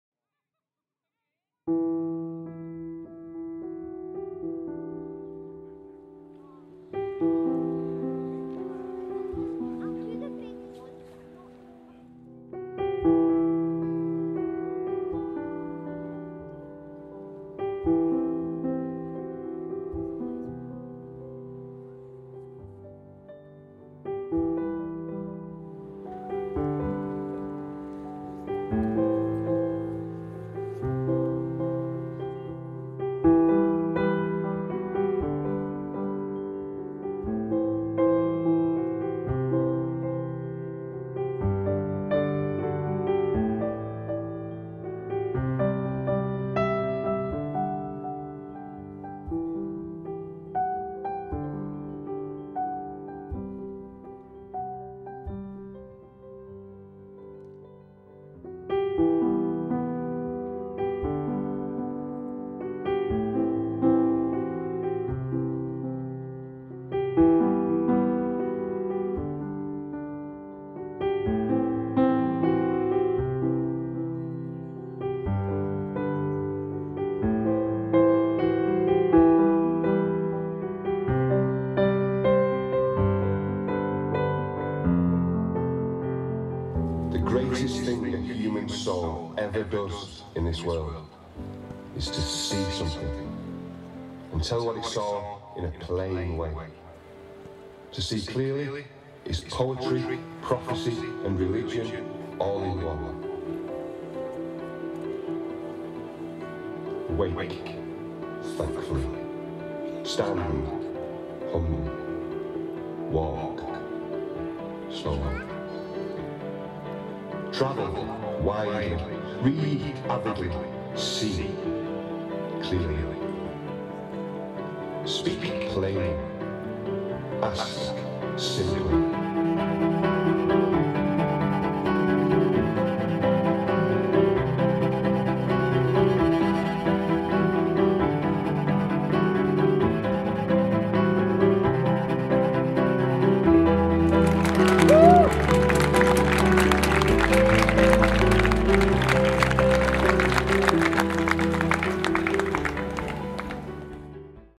Ruskin Museum Makeover at Meersbrook Hall, launch 20.10.18
2.5 min edited film of an outdoor projection event, 20th October 2018, the launch of the Ruskin Museum Makeover at Meersbrook Hall, Sheffield, produced by Ruskin in Sheffield for the Guild of St George.
Funded by Heritage Lottery Fund.